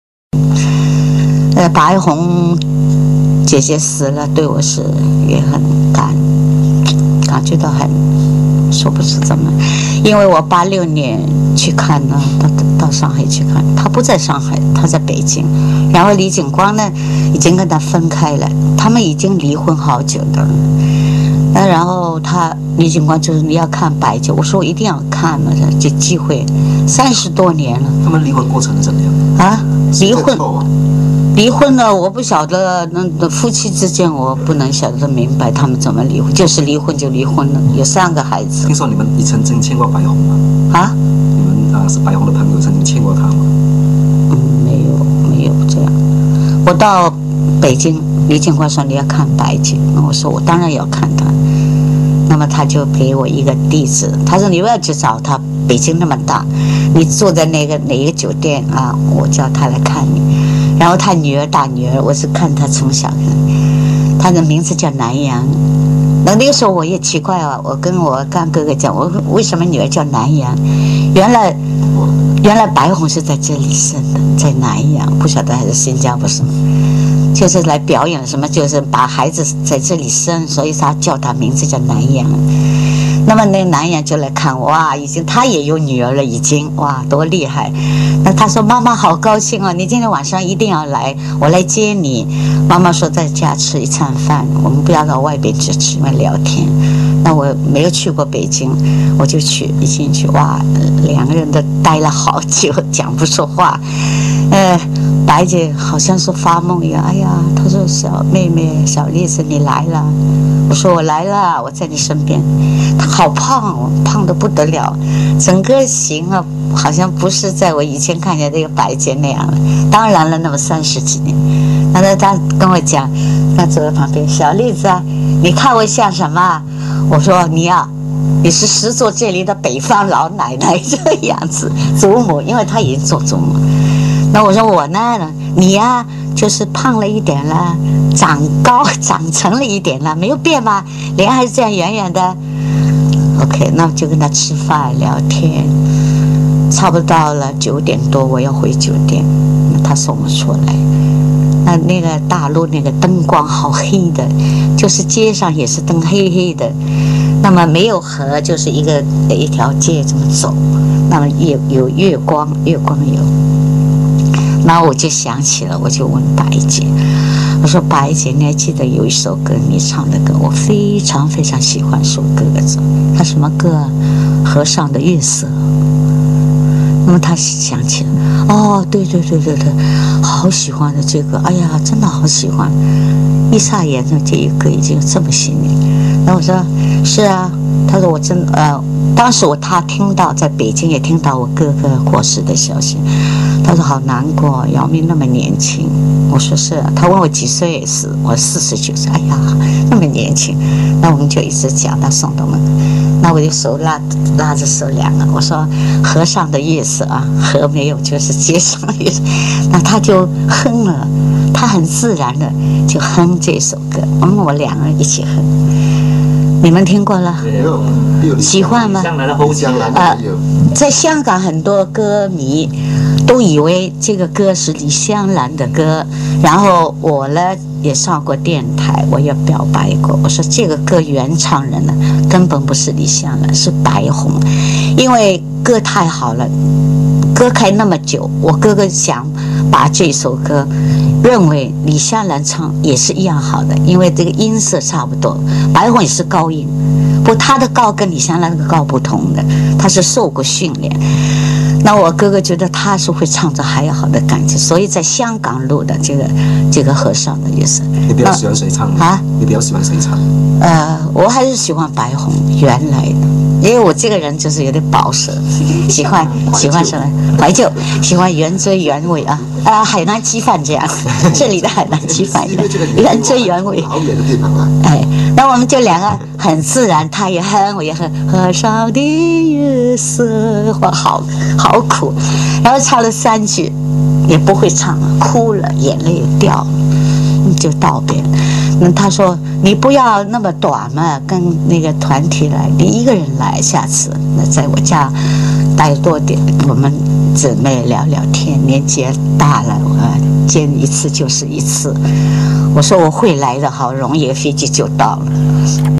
姚莉私人访谈录音片段-----谈白虹
在YOUTUBE有些老歌朋友喜欢白虹的歌曲， 再找来一些她的老歌来和大家分享之前，特意找出一段姚莉回忆当年（1986年）到北京探访白虹时的私人访谈录音。